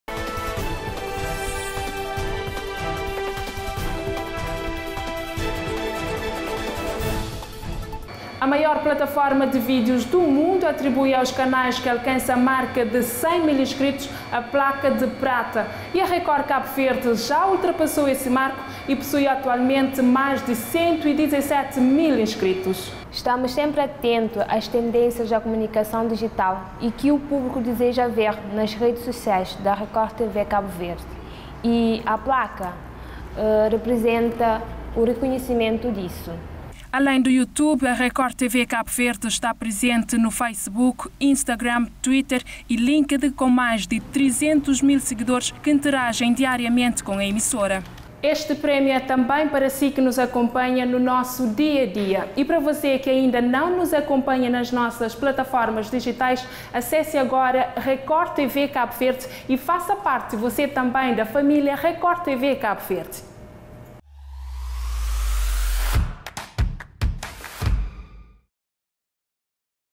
Record TV Cabo Verde ultrapassa o marco de 100 mil inscritos no Youtube e ganha placa de prata
A Record TV Cabo Verde tem mais de 32 milhões de visualizações no Youtube, sendo o canal de comunicação social mais acessado em Cabo Verde.